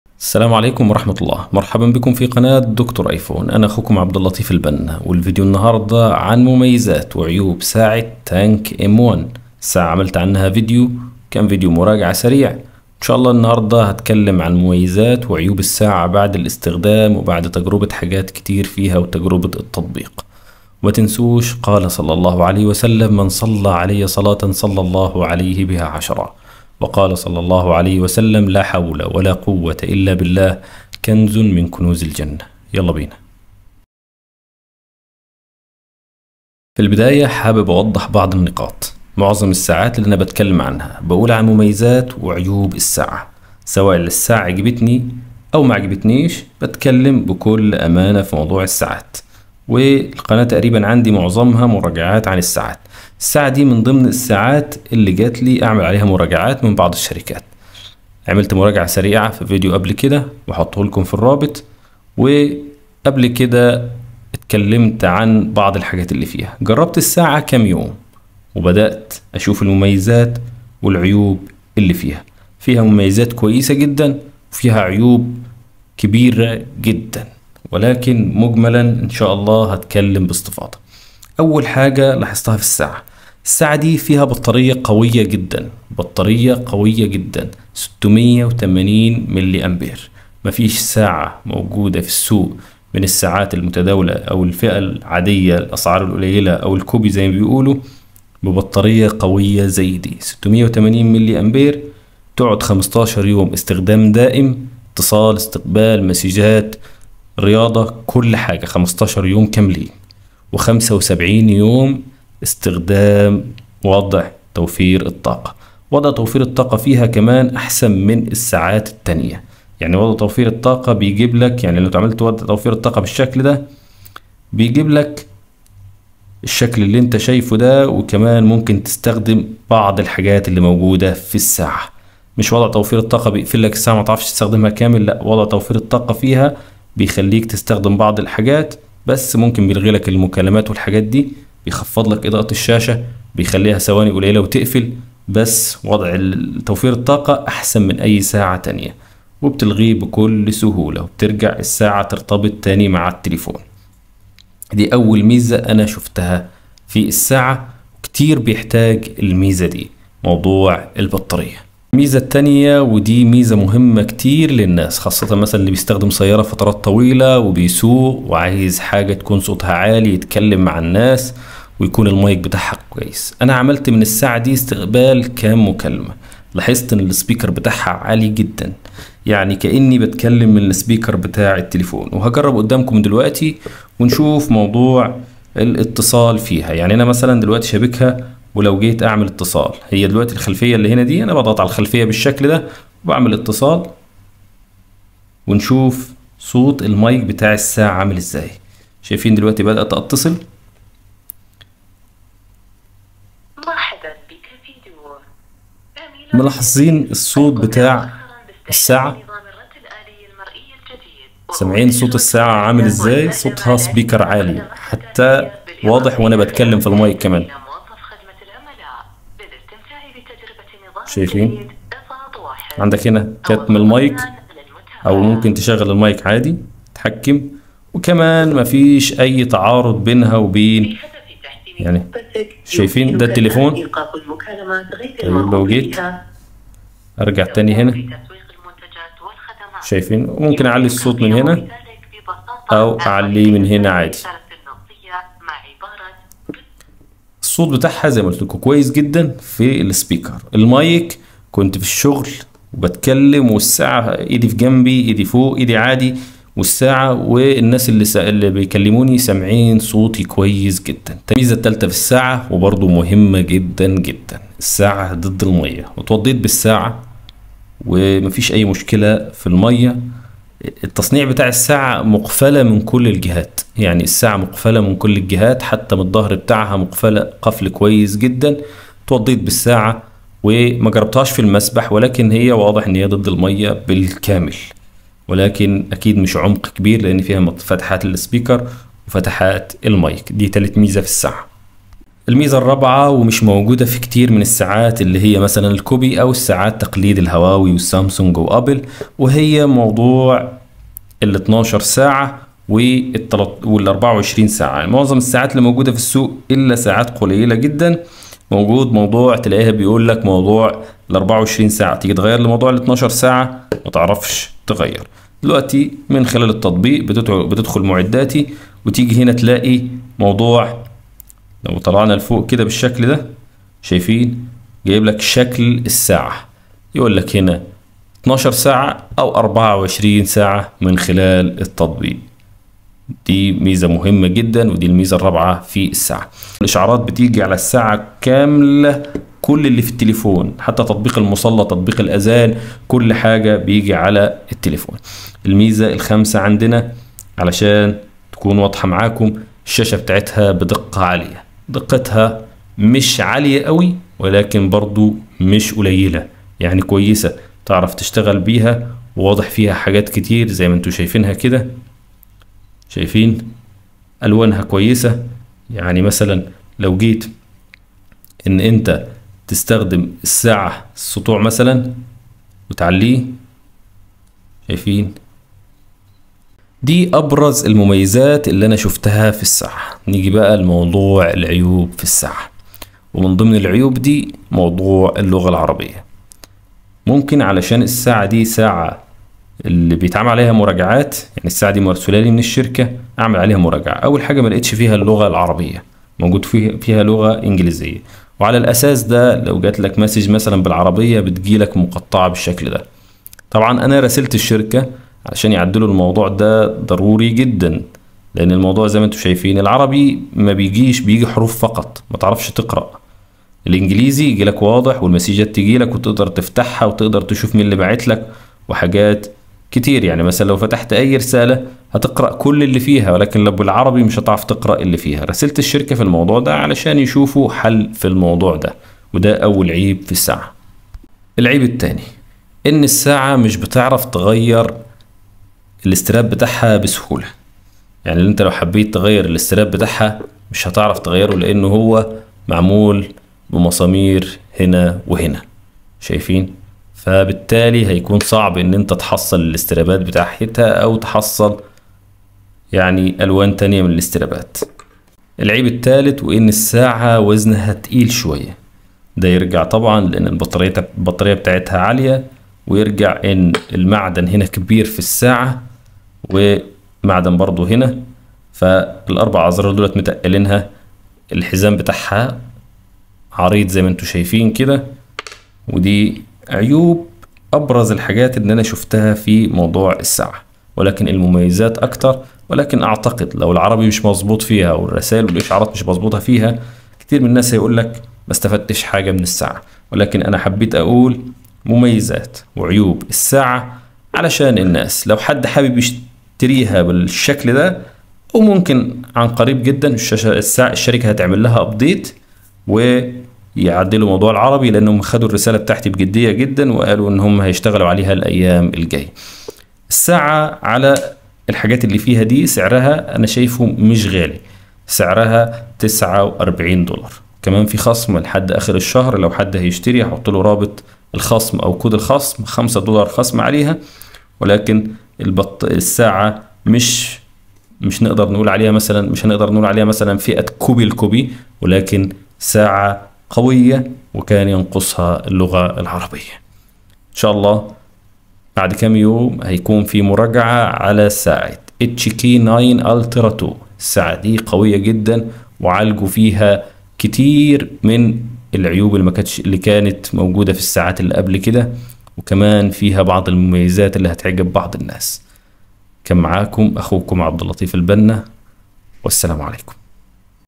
ساعة TANK M1 بطارية 75 يوم ، مميزاتها وعيوبها
ساعة كوبي قوية جدا بطارية قوية تدوم 75 يوم في وضع التوفير وسماعة قوية كأنها تلفون
ساعات ذكية / ساعات كوبي
ساعات ذهبيه

لشراء الساعة والحصول علي الخصم

The expiration time is 30th Nov.